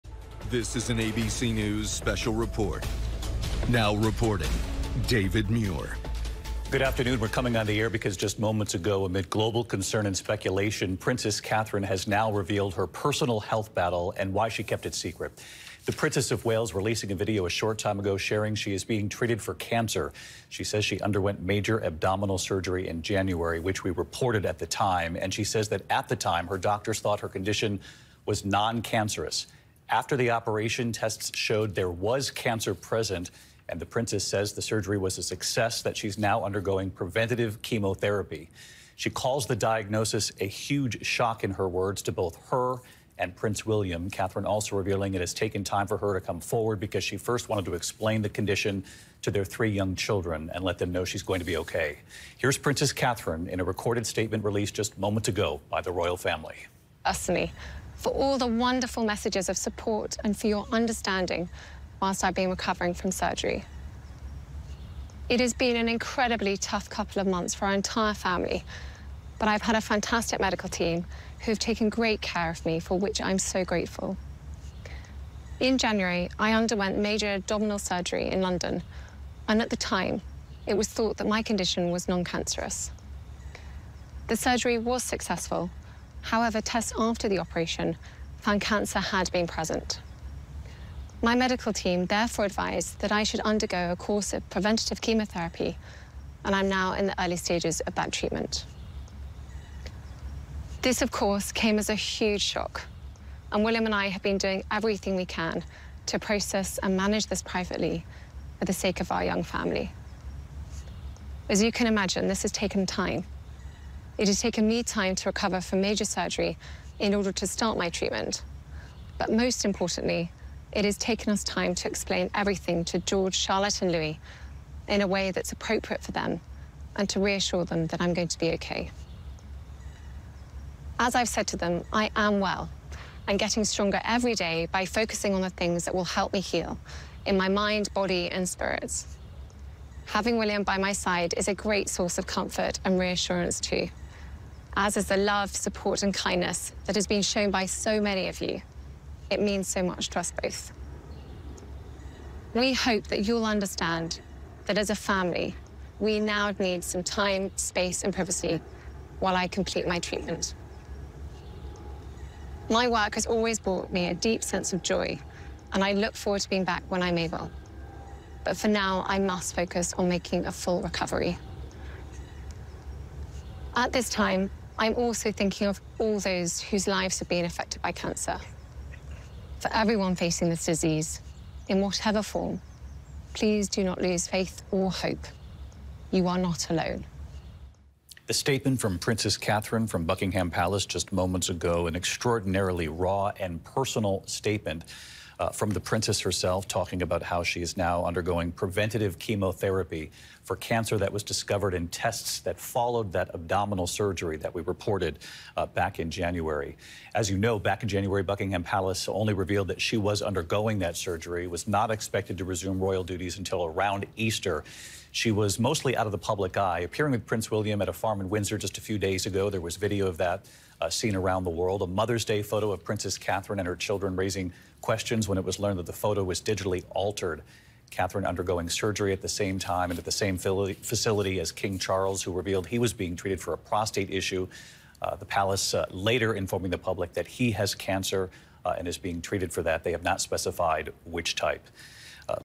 Kate Middleton says she was diagnosed with cancer, is undergoing chemotherapy
Catherine, Princess of Wales, was diagnosed with cancer and is undergoing chemotherapy, she said in a video message released on Friday.

The type of cancer has not been disclosed. She started chemotherapy in late February and the announcement comes as her father-in-law, King Charles, also undergoes cancer treatment.

"This of course came as a huge shock, and William and I have been doing everything we can to process and manage this privately for the sake of our young family," Kate, 42, said in the video, which was filmed at Windsor on Wednesday. "As you can imagine, this has taken time. It has taken me time to recover from major surgery in order to start my treatment."